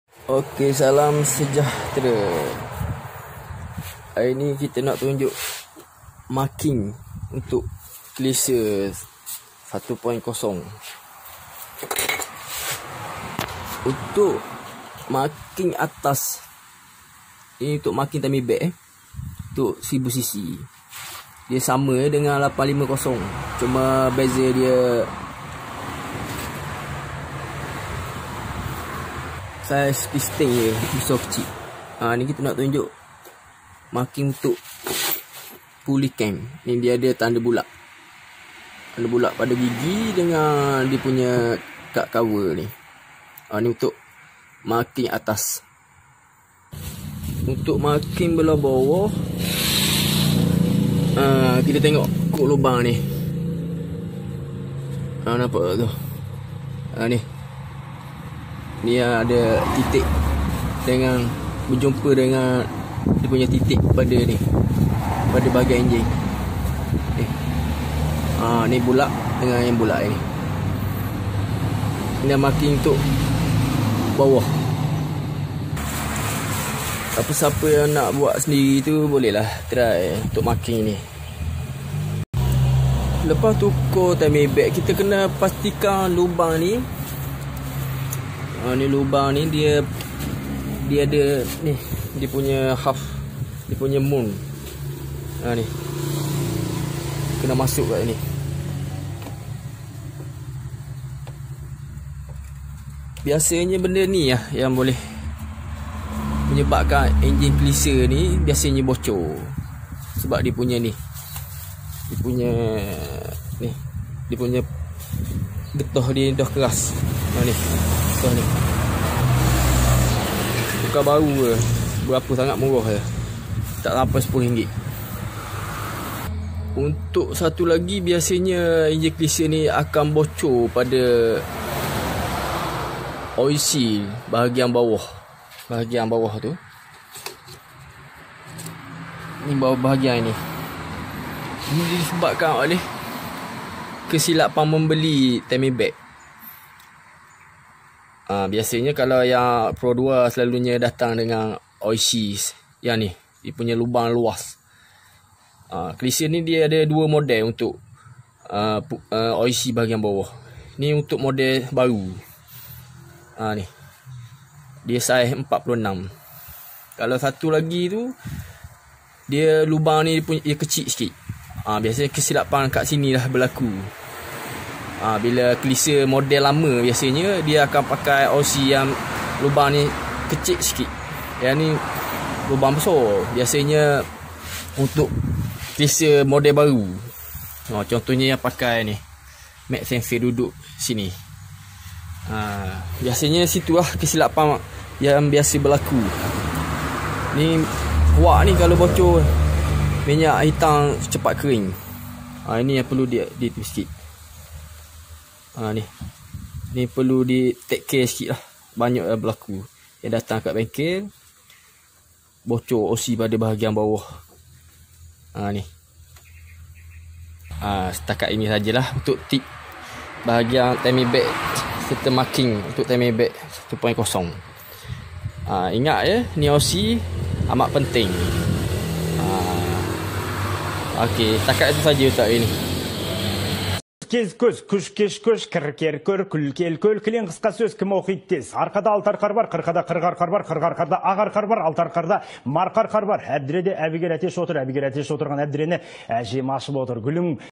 Timing mark kelisa,viva 1.0,850
(1)marking pully atas ialah titik bulat pada pully atas selari dgn tanda garis pada cover head,marking pully bawah titik pada pully bawah selari dgn titik kecik pada oilpump,Cara marking timing belt kelisa,viva Dengan Cara yg betol,marking timing hendaklah betol untuk pastikan enjin tidak semput atau lost power,Dan juga menyebabkan enjin rosak,valve terantuk din piston...
(2) semasa tukar timing belt pastikan juga periksa penutup lubang head atau full moon,benda ni biasanya getah keras dan tidak fit lagi menyebabkan keluar minyak hitam atau enjin leaking,selalu berlaku pada enjin kelisa,tak mahal rm10 sahaja,INI kerana untuk mengelakan terjadi kerja dua Kali...rugi upah pomen bukak tempat yg Sama bila Ada bocor.
(3)oil seal pully timing belt bawah,past ikan said yg betol SBB terdapat 2 saiz besar Dan Kecik,model kelisa lama pakai oilseal lubang kecik,model 2004 keatas pakai lubang besar,semasa membeli pastikan saiz yg betol ,kerana silap saiz akan menyebabkan berlaku kebocoran yg besar,Dan leaking yg Kuat ,minyak Hitam begitu CEPat Kering

Sekian tip untuk Kali ini
Ada ape2 pertanyaan sila KOmen Kat bawah